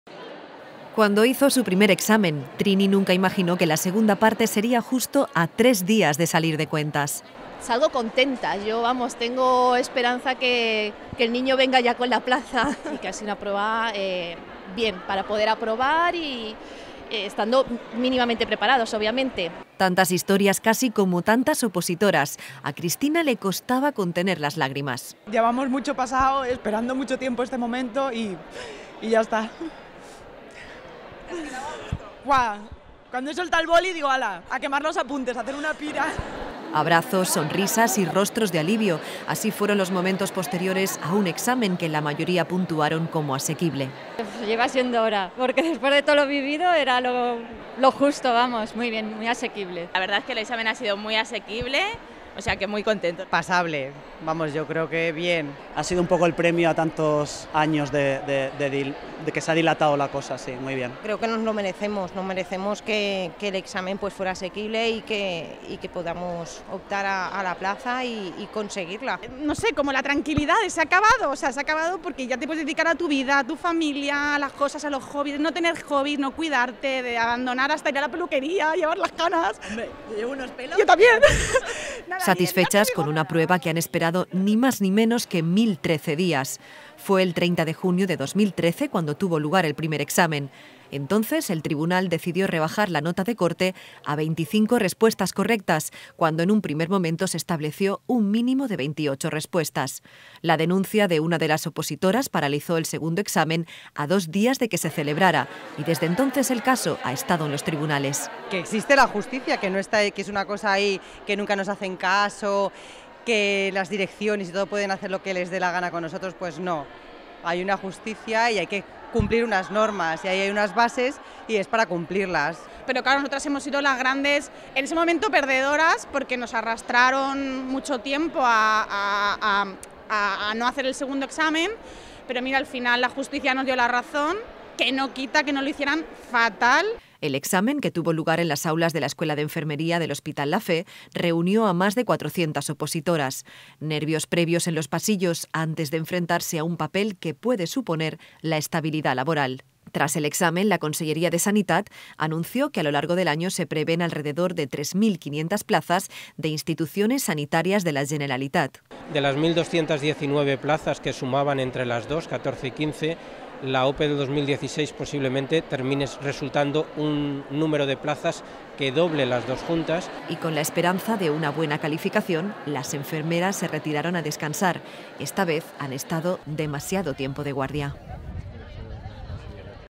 2º Examen OPE 2007 | Cecova TV
Han sido casi 2 años esperando para poder realizar el segundo ejercicio de una oposición que les puede abrir las puertas a la estabilidad laboral. Más de 400 enfermeras se presentaron este domingo 13 de marzo al segundo examen de la OPE 2007. Una oposición que, hasta ahora, ha estado en manos de los tribunales. Acabada la prueba las opositoras se han mostrado satisfechas y esperanzadas.